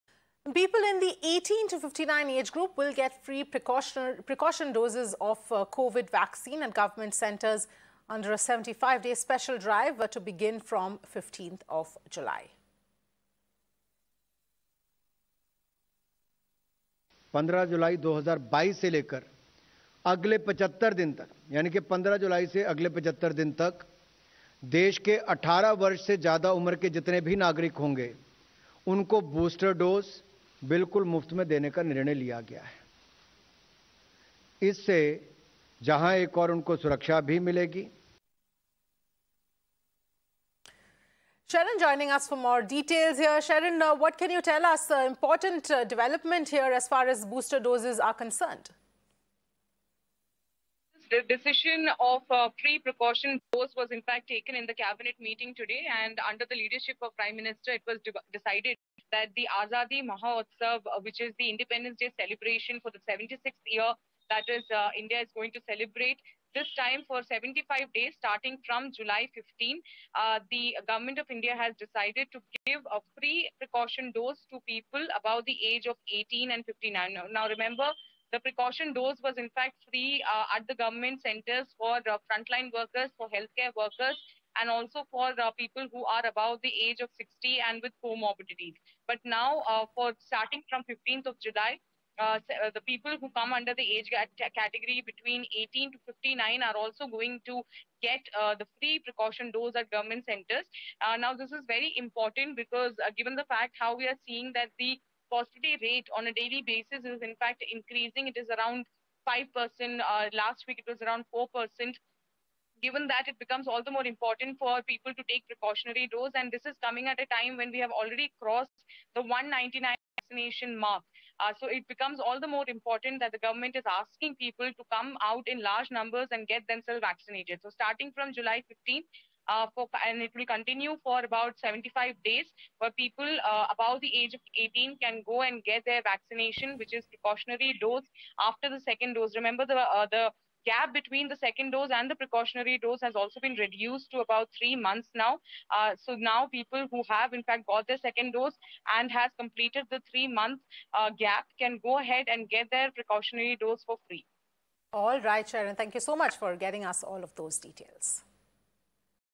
Free Jabs For 18+ Age Group | Special Drive For Booster Doses | Breaking News
According to the latest updates on the Covid Booster doses, people in the 18 to 59 age group will get free booster doses of Covid vaccines. The government has started a special drive from July 15. According to the sources, the decision on the special vaccination drive was taken in the cabinet today.  The decision was taken under the leadership of Prime Minister Narendra Modi. On the occasion of  Azadi Mahotsav which is the Independence day celebration for the 76th year, the Indian government has decided to give free precaution doses to people above the age of 18 years. Watch the entire video to know more.